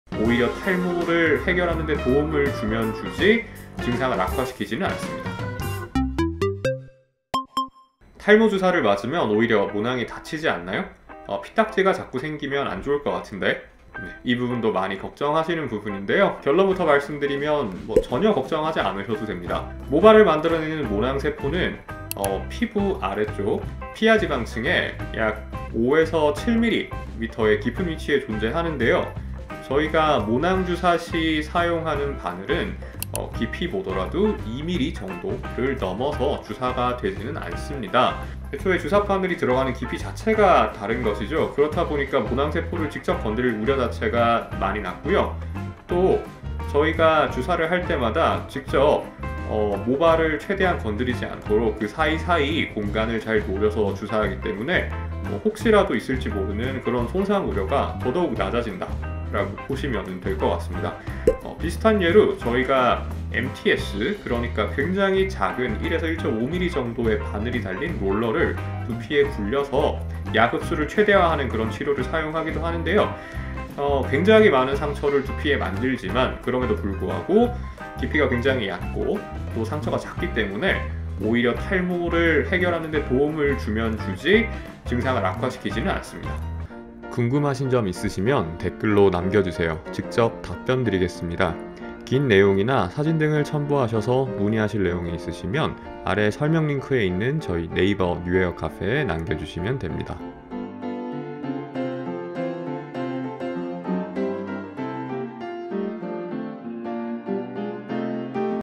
탈모 주사, 오히려 모낭을 다치게 하진 않을까? l 뉴헤어
탈모 주사가 모낭을 다치게 한다?!
주삿바늘 때문에 오히려 모낭이 다치지 않을까 하는 걱정인데요
걱정 NO NO!! 탈모 주사치료는 안전합니다 :)

오늘 영상으로 궁금증이 해결되셨길 바라며

다른 영상 구경하러 가기🙌